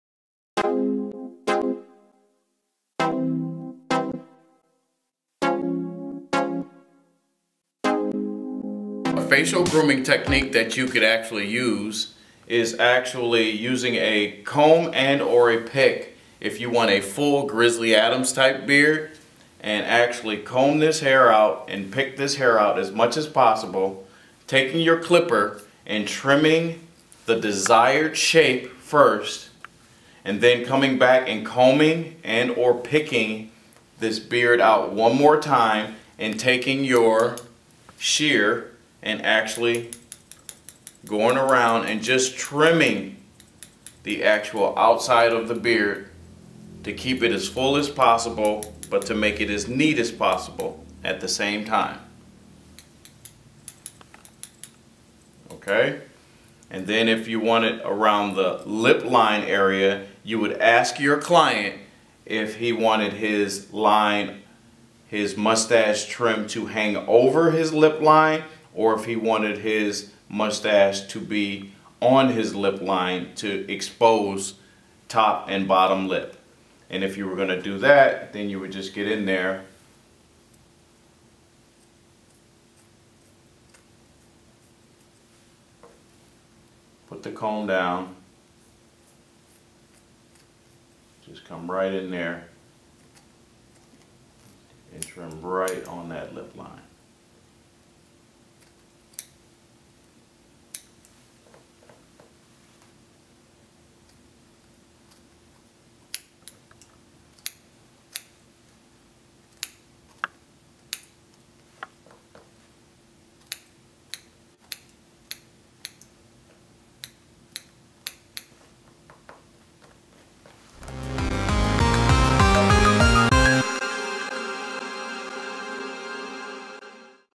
Facial Hair Grooming Technique for Barbers
Grooming hair on the head is one thing, facial hair grooming is a whole other area that requires different techniques and know how. When it comes to mustaches and beards, shaping a them can be a bit tricky. Here's a facial grooming technique for mustaches and beards by Chris Vaughn, the Beverly Hills Barber.